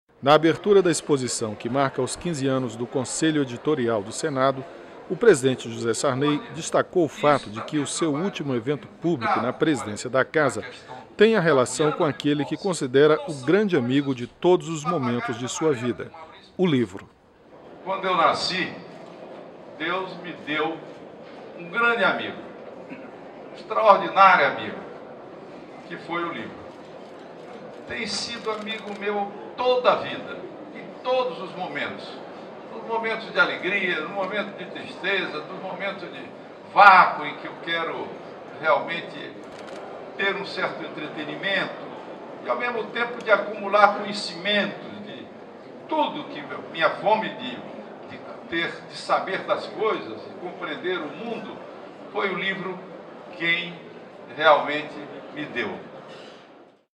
Sarney participa de último evento público na presidência do Senado
Sarney, que preside o conselho desde 2002, disse que foi uma feliz coincidência fechar seu ciclo à frente da Casa com um evento dedicado ao livro.